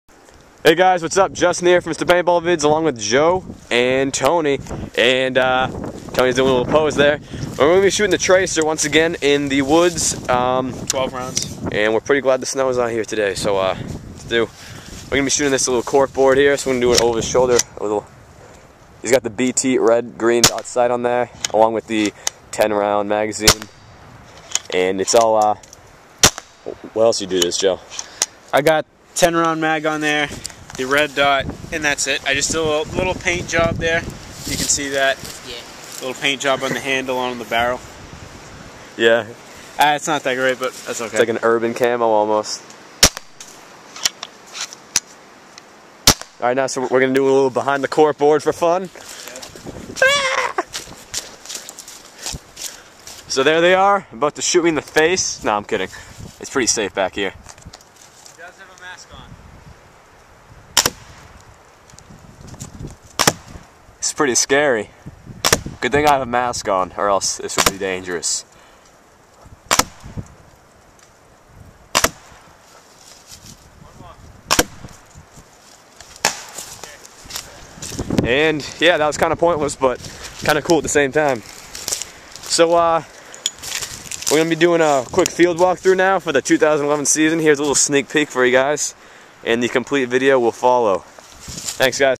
Empire Trracer pump paintball gun shooting
A quick shooting video of the Empire Trracer pump paintball marker.

This video clip is pretty old, from before we got the Apex 2 barrels. Just had it saved on my computer and thought you guys might like to see it.